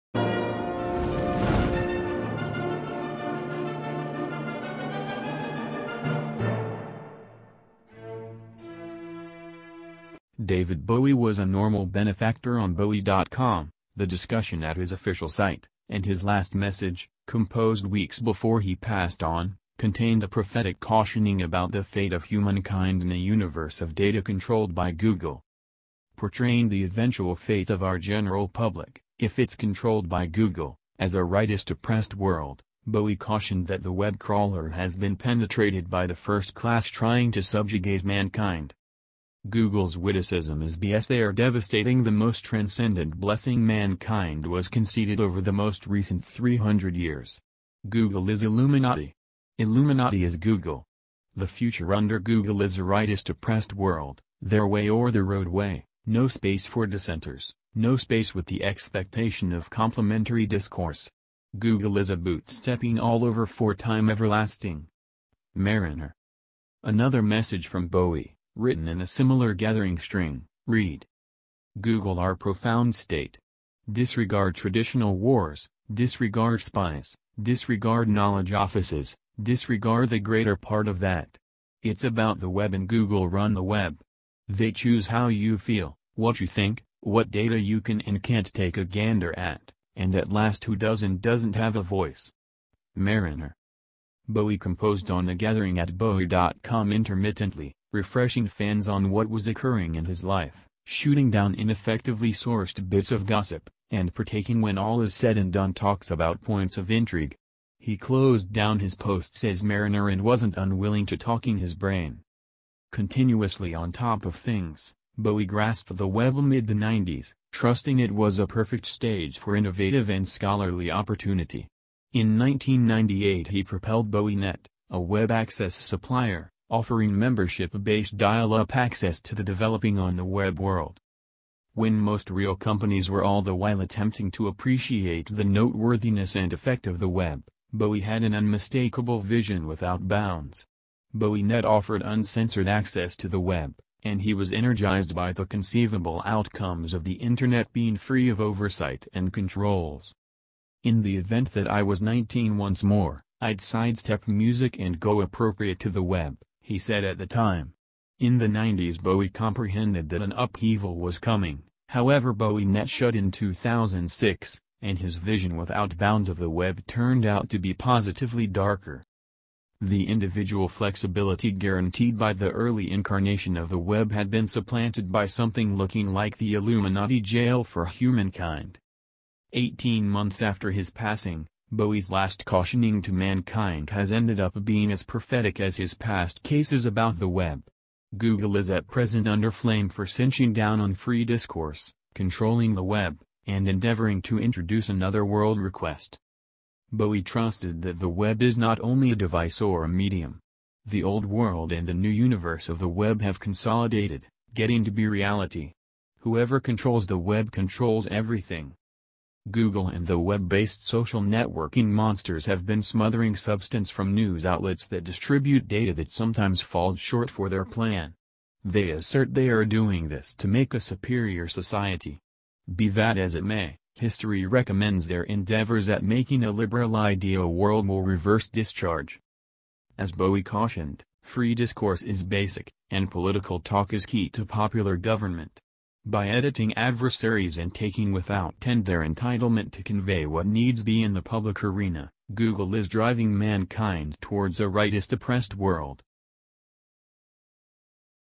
David Bowie’s Last Online Post Before Death ‘Google Is Illuminati’
David Bowie was a normal benefactor on bowie.com, the discussion at his official site, and his last message, composed weeks before he passed on, contained a prophetic cautioning about the fate of humankind in a universe of data controlled by Google.